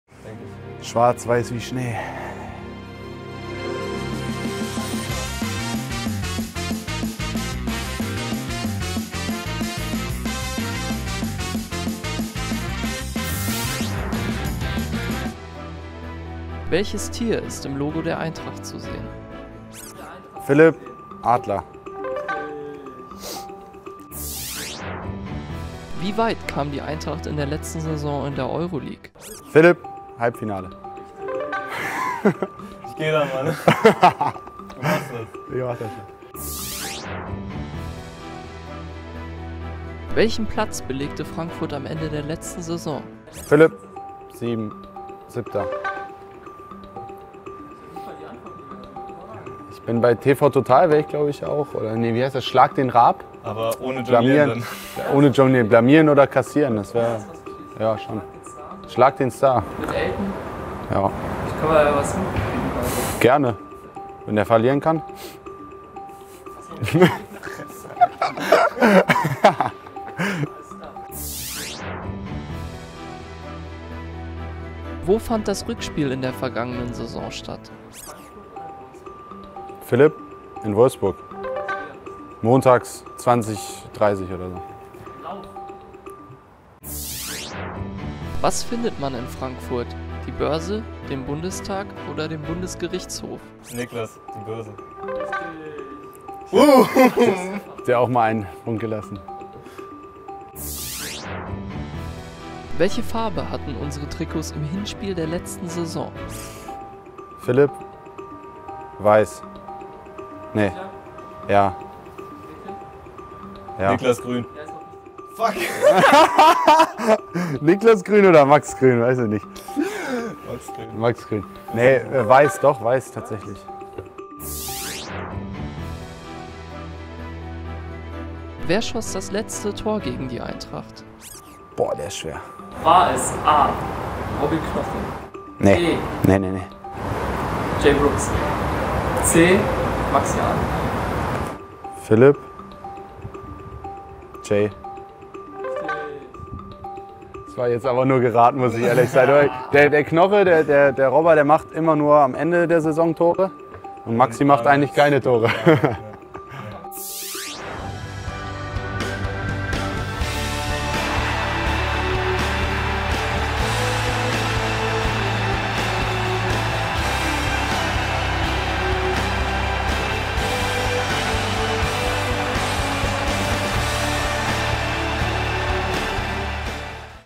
Niklas Klinger vs. Phillip Menzel | Spieltagsquiz vor Eintracht Frankfurt - VfL Wolfsburg
Im Spieltagsquiz vor dem Auswärtsspiel bei Eintracht Frankfurt treten die beiden Nachwuchstorhüter unserer Wölfe Niklas Klinger und Phillip Menzel gegeneinander an!